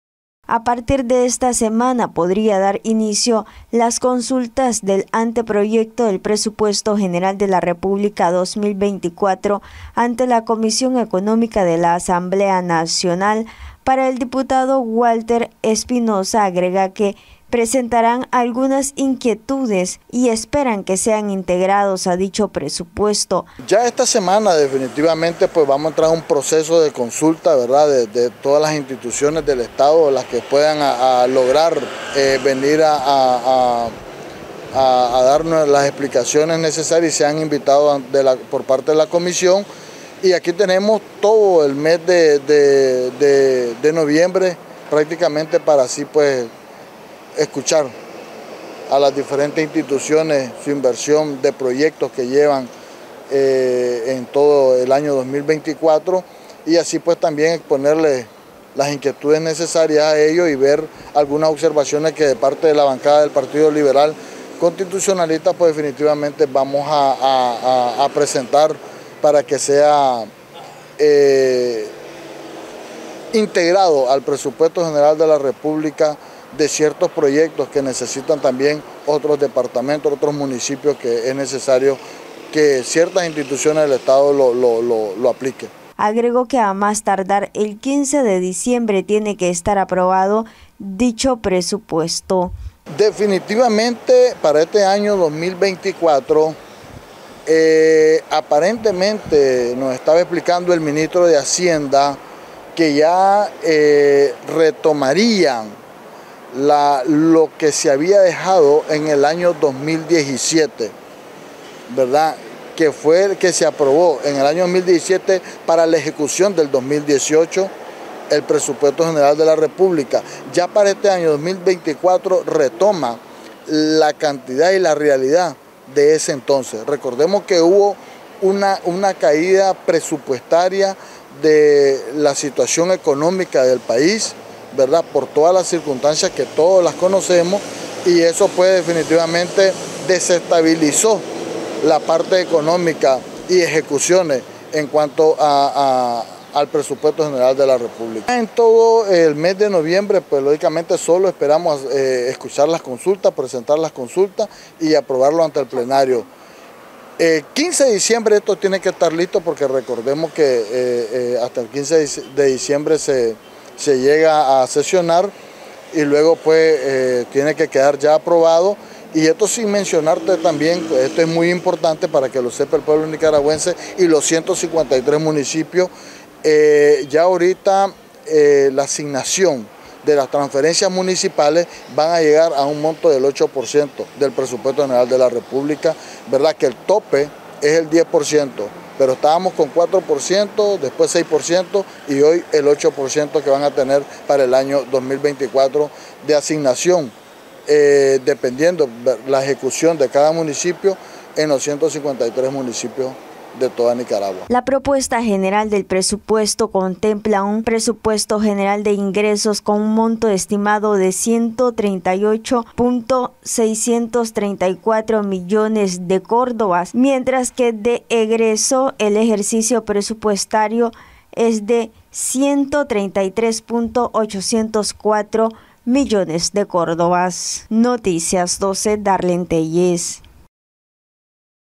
Noticias12Nicaragua En esta semana podría dar inicio el proceso de consultas del PGR 2024
El diputado Walter Espinoza dio a conocer que a partir de esta semana podría iniciar el proceso de consultas del Presupuesto General de la República 2024